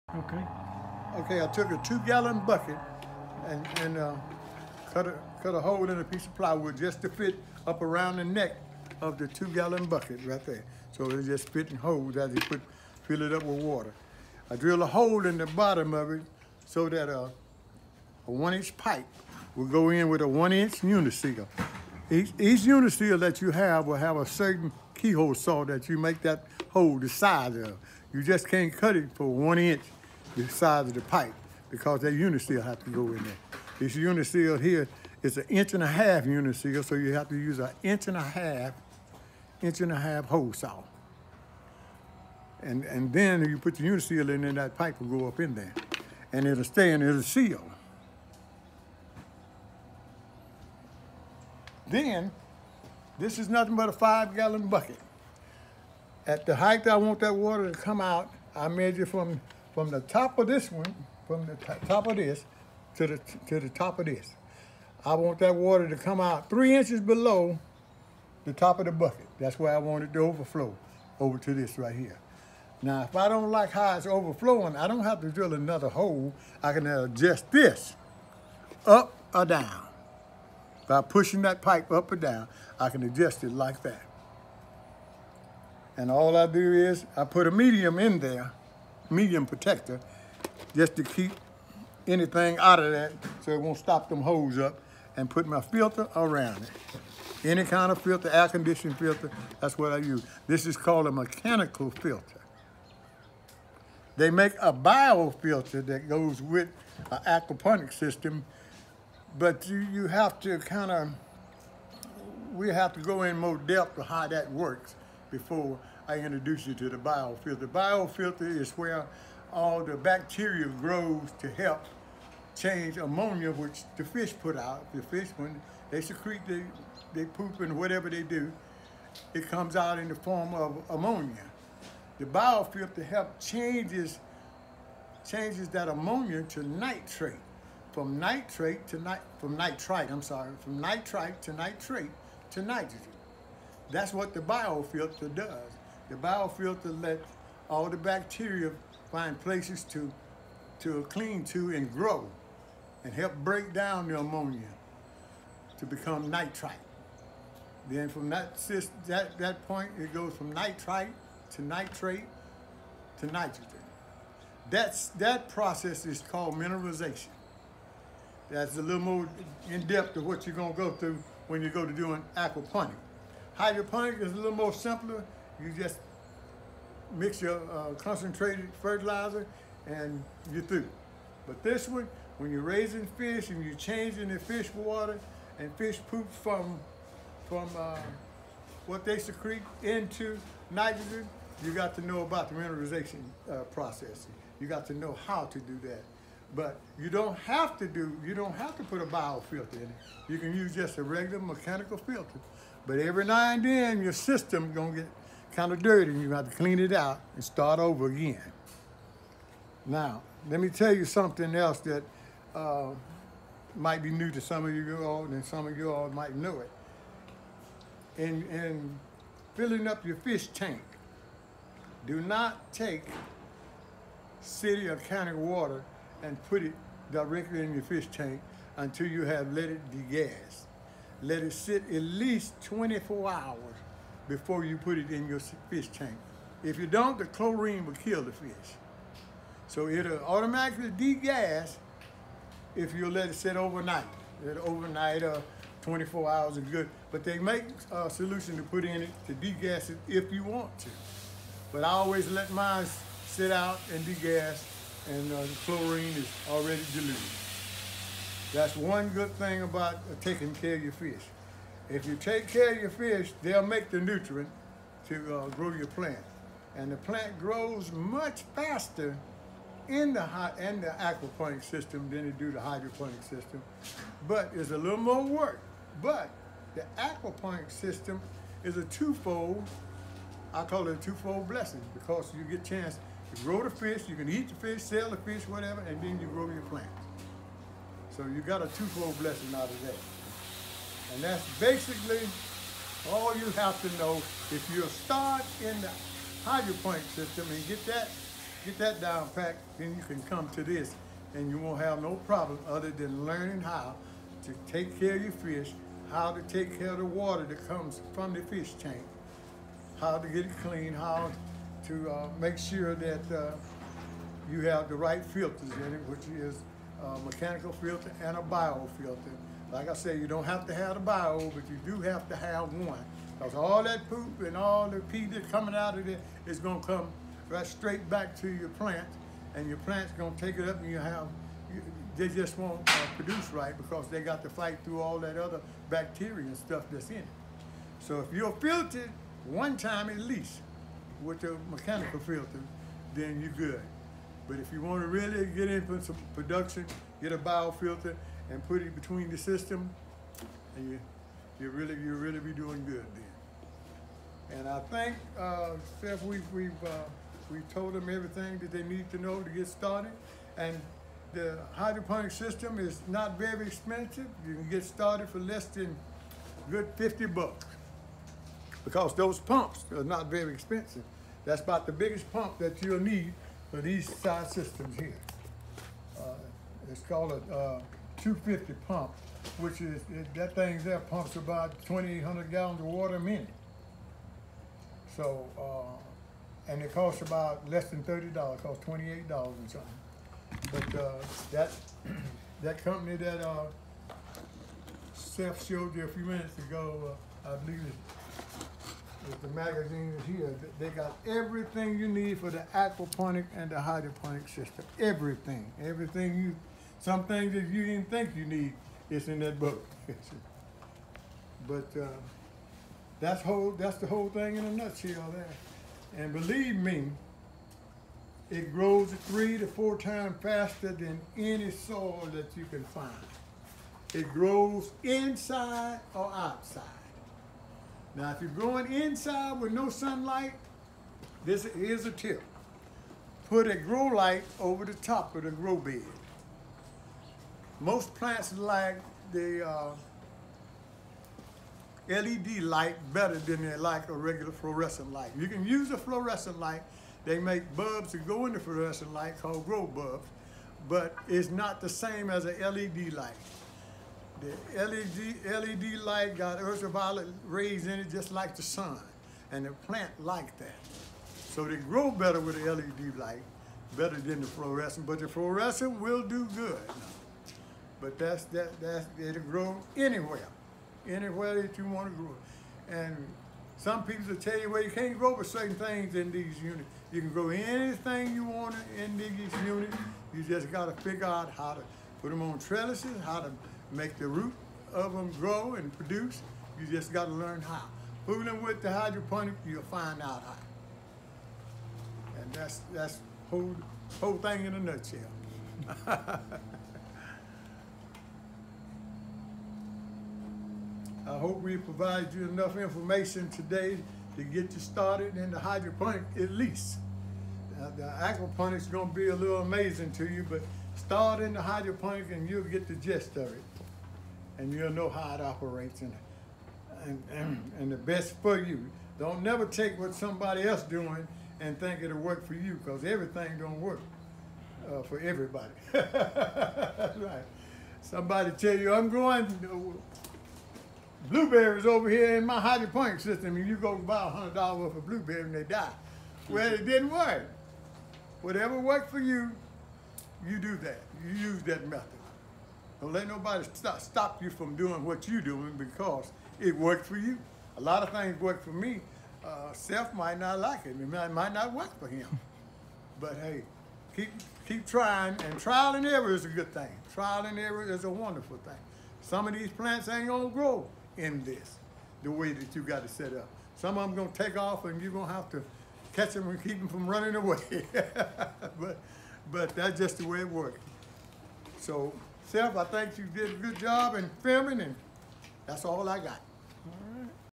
Introduction to Hydroponics Workshop (4)
This is Part 4 of a 4-Part series in which Dr. Sherman Green of Athens Land Trust provides an overview of a low-cost, home-scale hydroponic grow system.  In this video, Dr. Green provides more detail on constructing a small-scale aquaponic system.  This series is part of a Farmer Outreach workshop conducted by Athens Land Trust on Dec. 9. 2021.